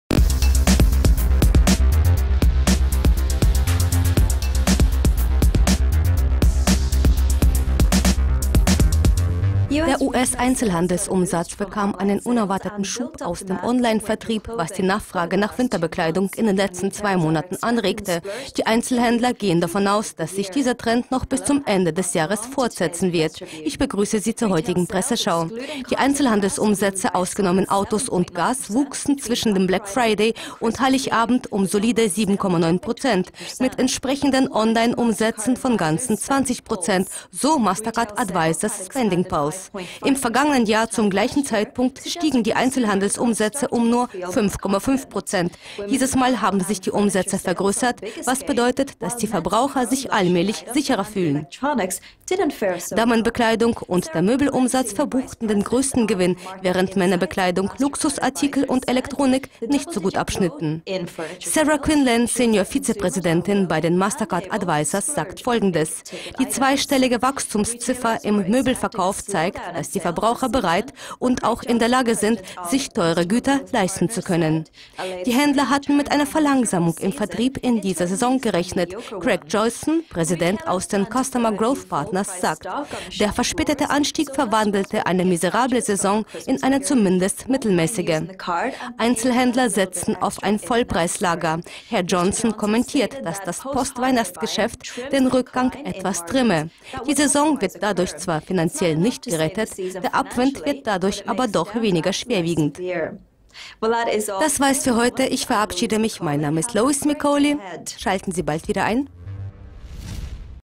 US-Einzelhandelsumsätze steigen
US-Einzelhandelsumsätze verzeichneten einen unerwarteten Schub aus dem Verkauf von Möbeln und Frauenbekleidung, was darauf hindeutet, dass die Verbraucher stetig zuversichtlicher werden.